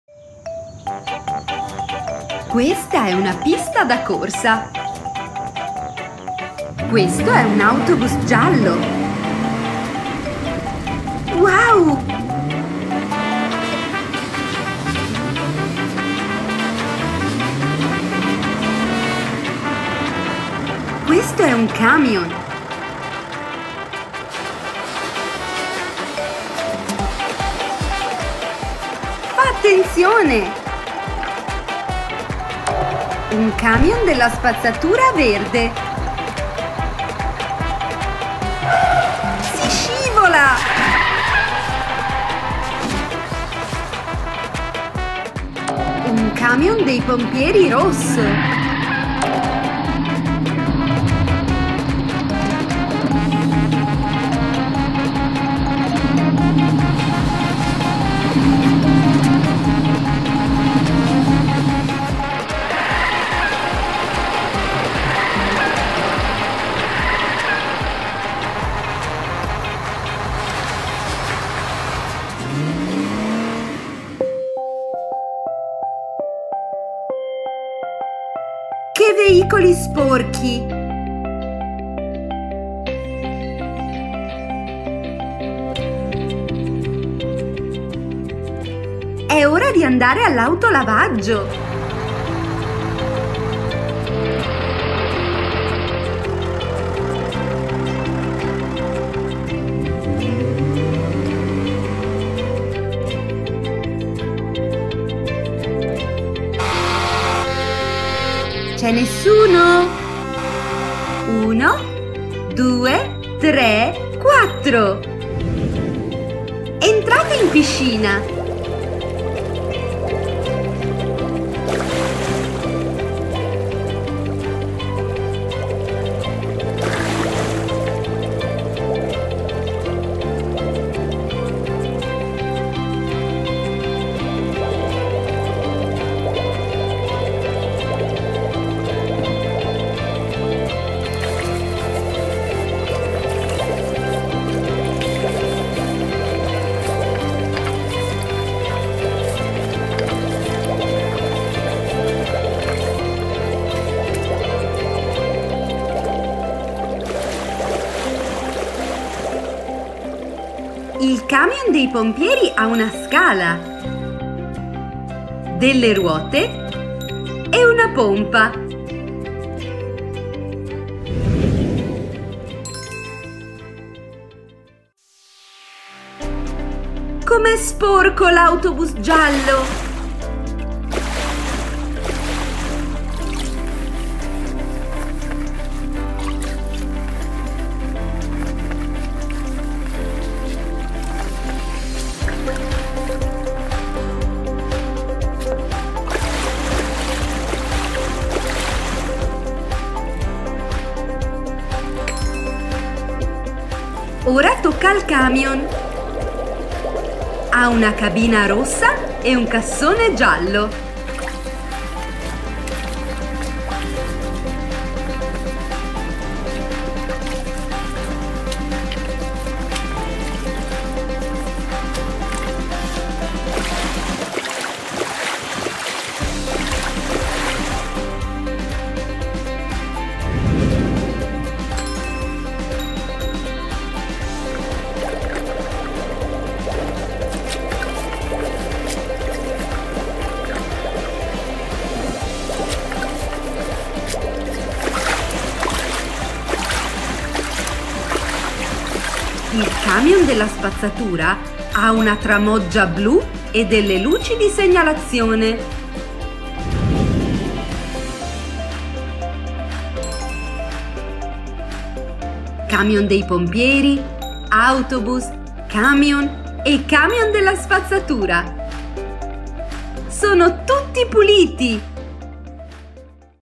Gara delle macchine giocattolo. Macchinine si divertono sulla pista da corsa. Giochi per bambini
Vediamo la gara delle macchine giocattolo nei nuovi giochi per bambini! Guarda, le macchinine si divertono sulla pista da corsa. Ecco, ci sono l’autobus giallo, il camion, il camion della spazzatura verde, il camion dei pompieri. Fate attenzione! Si scivola! Ci sono delle pietre grandi e della sabbia. Ora le macchinine sono tutte sporche. Dai, portiamole all’autolavaggio. Macchinine, entrate nella piscina! Puliamole bene con una spazzola. Adesso sono tutte belle pulite. Contiamo le nostre macchine giocattolo insieme: una, due, tre, quattro. L’autobus, il camion, il camion della spazzatura e il camion dei pompieri.